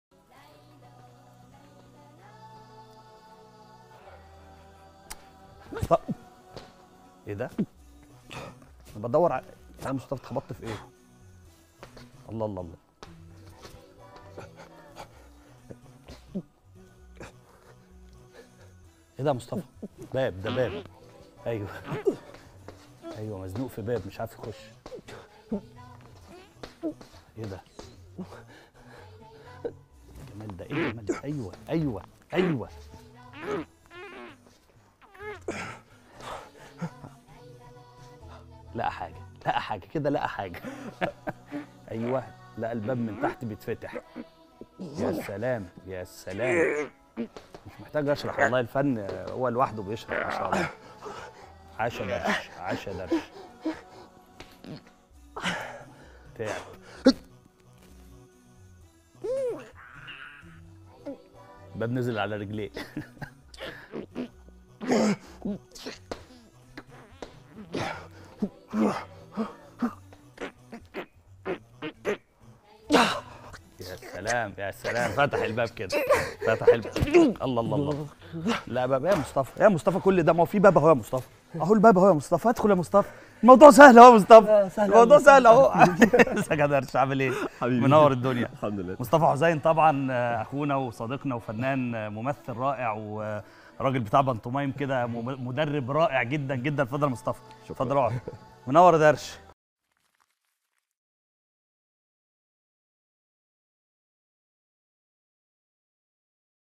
مشهد تمثيلي صامت رهيب! 😯 أبدع فيه الممثل مصطفى حزين ✨
فنون ومهارات إعلامية
نتعلمها مع خبراء في هذا المجال

تقديم: محمد غزي
ضيف الحلقة: الممثل مصطفى حزين

تابعنا على:
• تويتر (إكس)

ـــ
شبكة المجد ..
13 قناة آمنة لأسرتك!